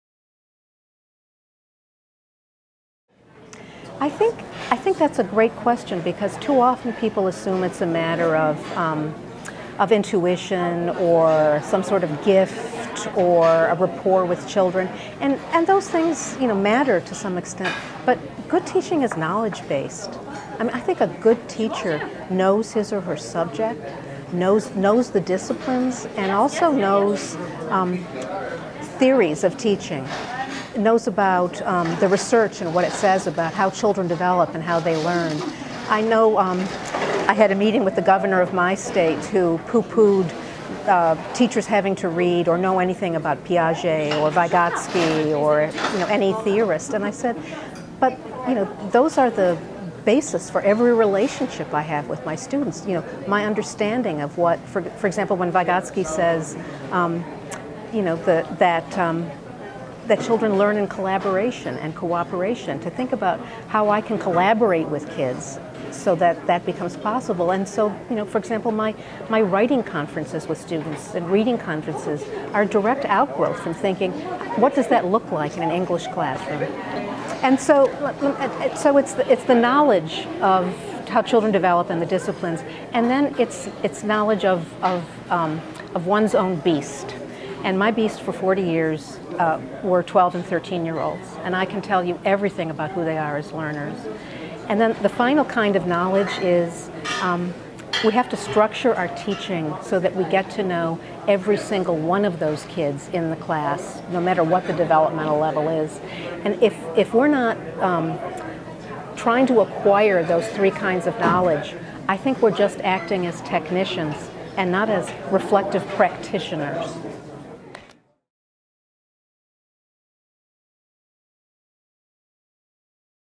Nancie Atwell on what makes an effective teacher
Nancie engages in a Q&A about education in this conversation that took place before her Clinton Global Initiative and Varkey Foundation Topic Dinner speech celebrating educators. Read the blog post